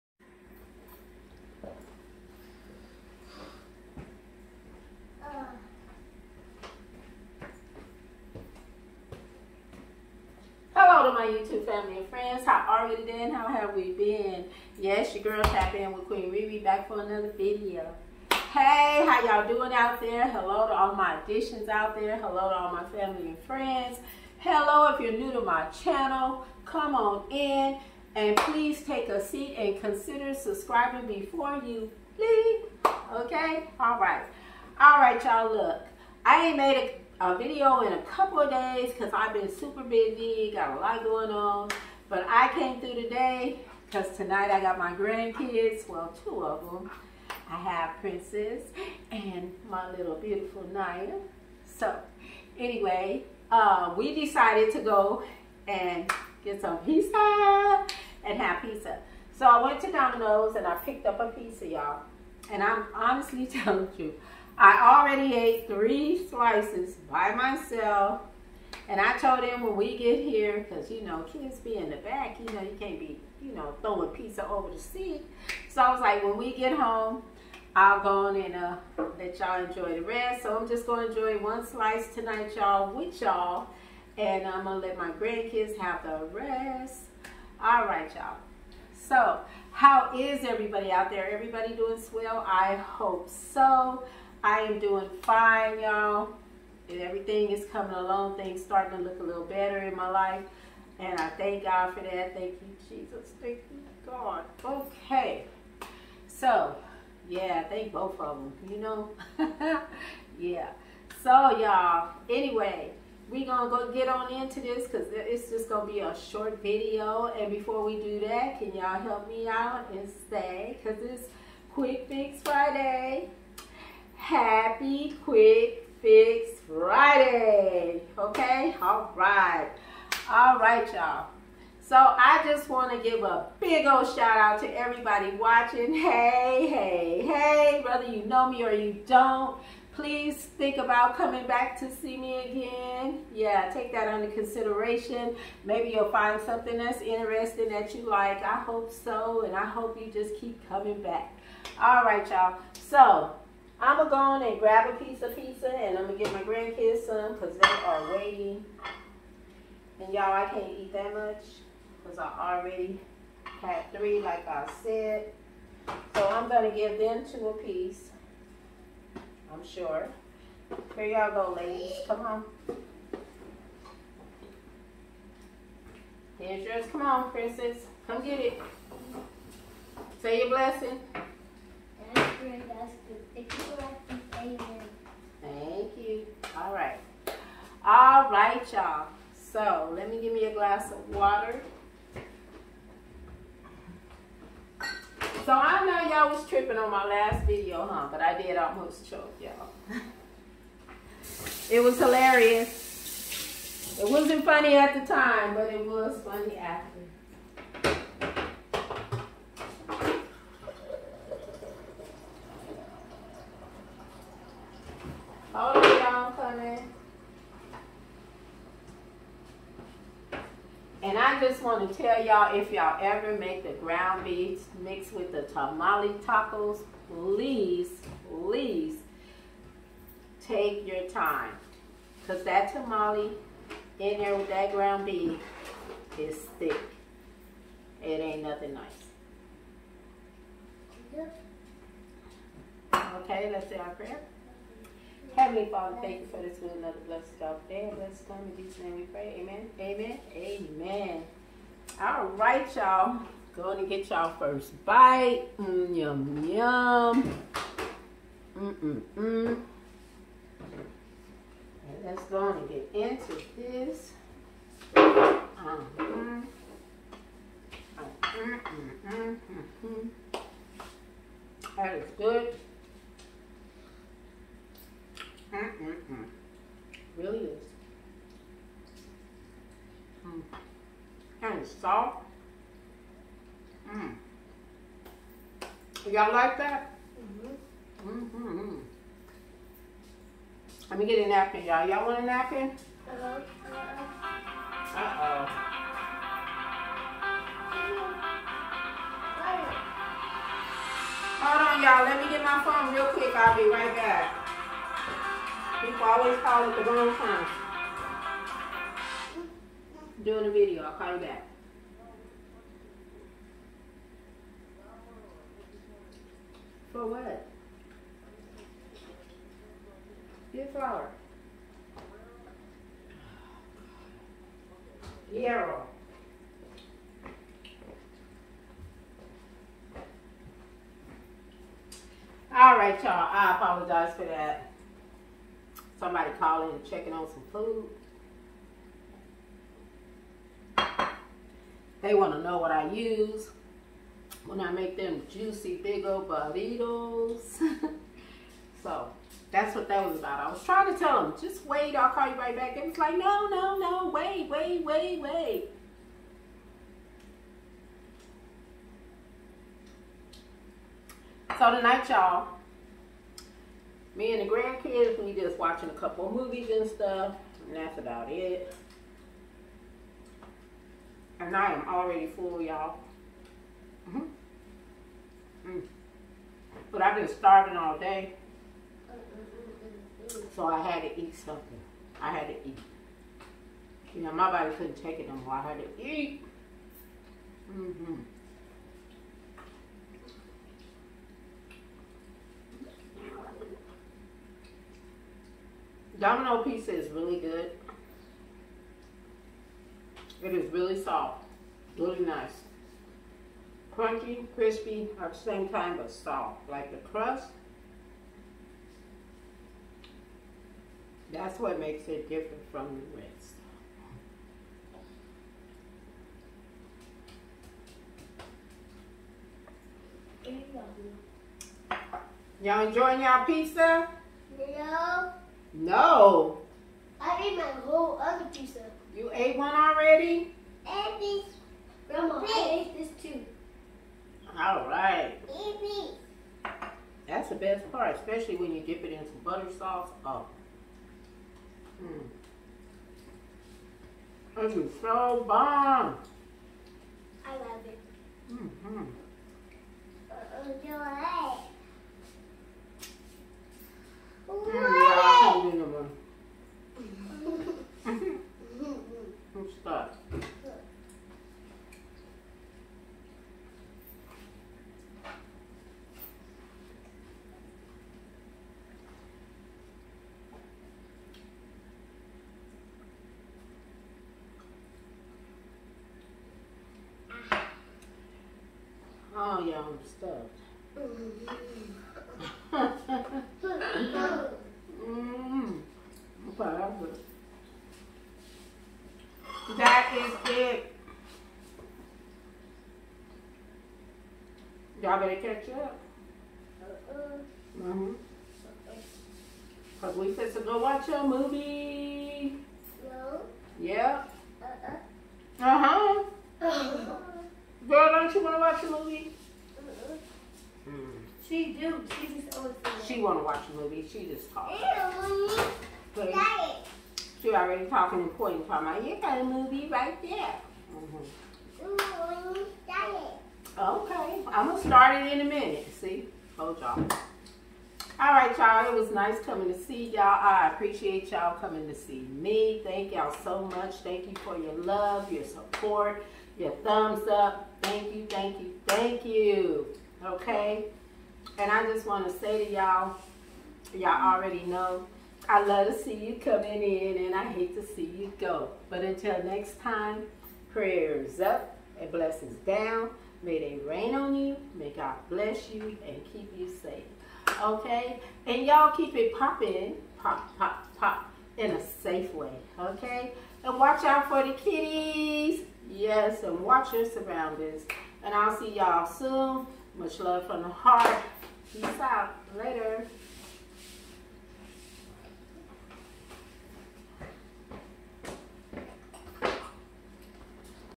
DOMINOES PEPPERONI PIZZA, QUICK FIX FRIDAY 😋😋
Today I got it Popp'in with some Domino's Pizza. Much Love Queen Ree Ree 🤗❤

FAN MAIL:
                    REE REE JO
                    P.O.BOX961
          EL MIRAGE,  ARIZONA
                         85335